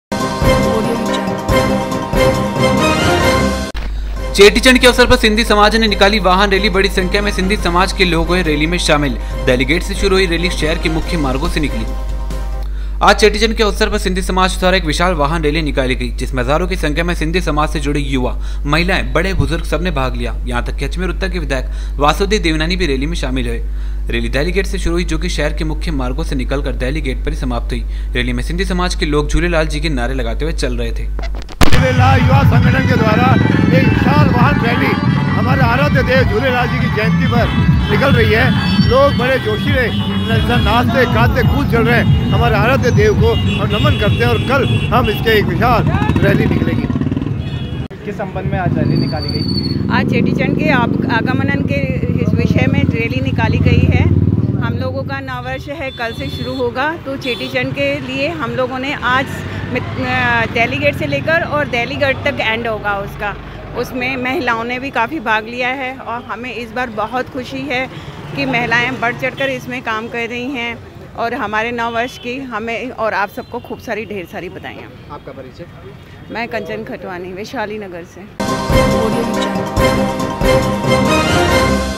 HORIZON HIND NEWS - चेटीचंड के अवसर पर सिन्धी समाज ने निकाली वाहन रेली
चेटीचंड के अवसर पर सिन्धी समाज ने निकाली वाहन रेली | बड़ी संखिया में सिन्धी समाज के लोग हुए रेली में शामिल | देहली गेट से शुरू रेली निकली शहर के मुख्य मार्गो से |

सिटी रिपोर्ट |

आज चेटीचंड के अवसर पर सिन्धी समाज द्वारा एक विशाल वाहन रेलों निकाली गई | जिसमे हजारो की संखिया में सिन्धी समाज से जुड़े युवा महिलाए बड़े बुजुर्ग सबने भाग लिया यहाँ तक की अजमेर उत्तर के विधायक वासुदेव देवनानी भी रेली में शामिल हुए | रेली डेल्ही गेट से शुरू हुई जो की शहर के मुख्य मार्गो से निकल कर डेल्ही गेट पर ही समाप्त हुई | रेली में सिन्धी समाज के लोग झुलेलाल जी के नारे लगाते हुए चल रहे थे |